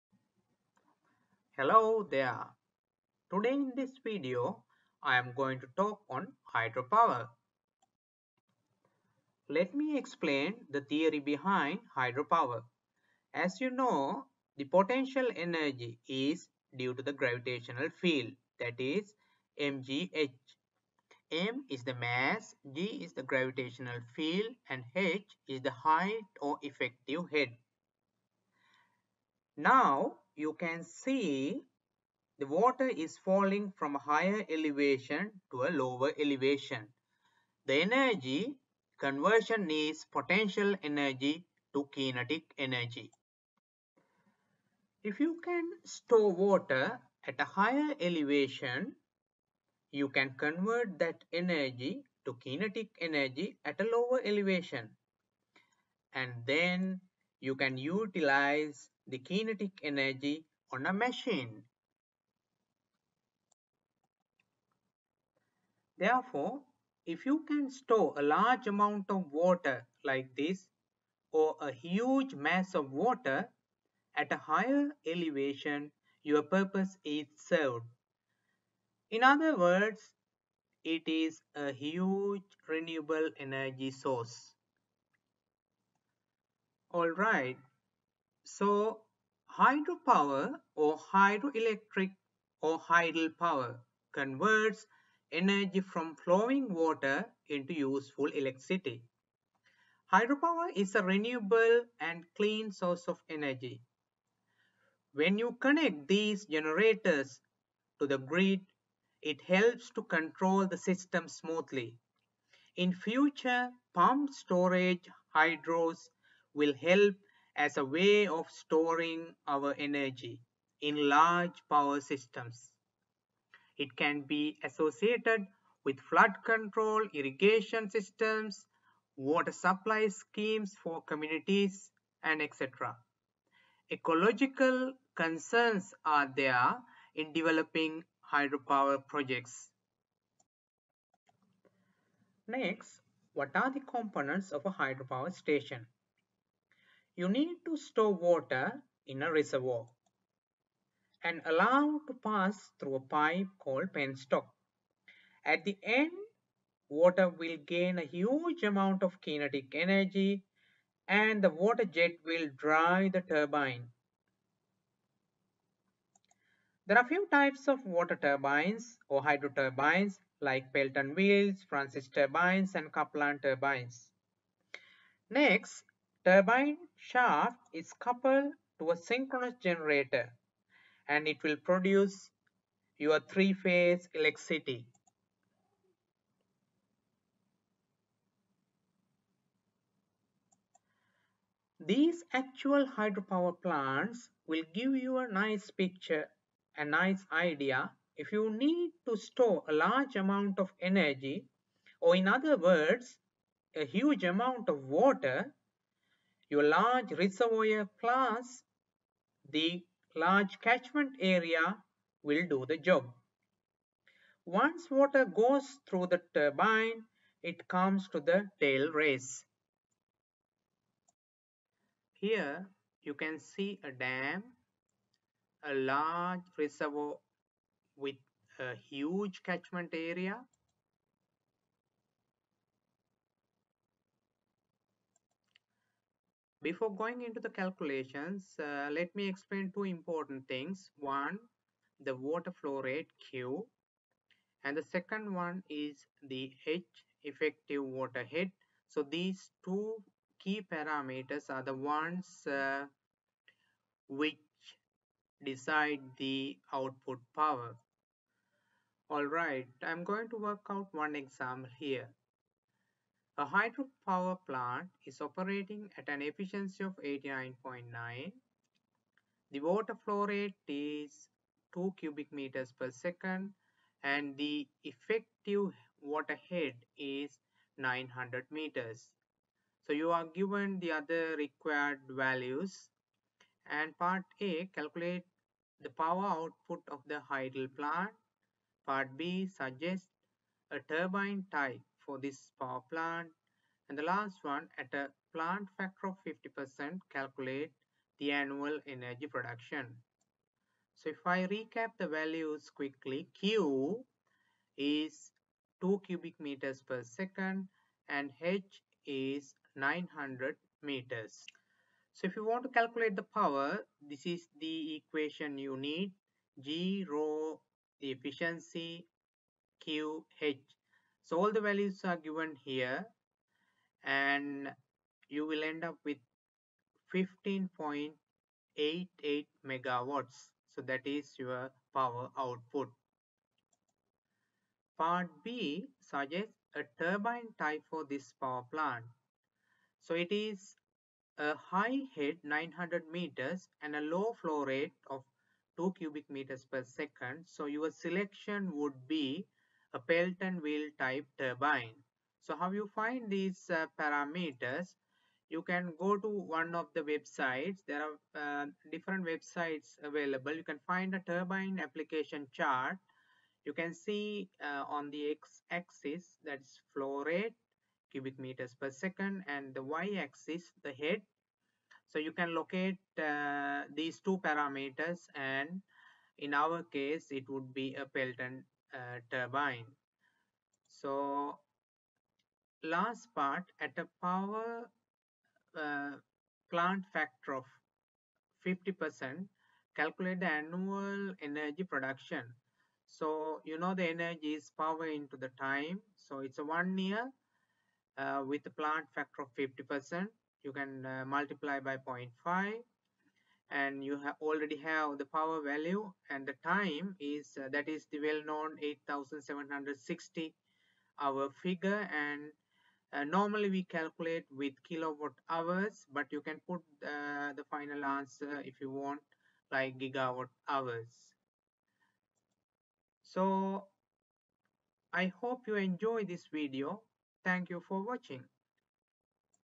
Hydropower | Hydroelectric | Hydel | Solved Example with Calculations
This video explains the theory behind Hydropower or Hydroelectric or Hydel power. As an example, one problem solving type question is taken and explained.
By Lanka Udawatta, PhD (Saga, Japan)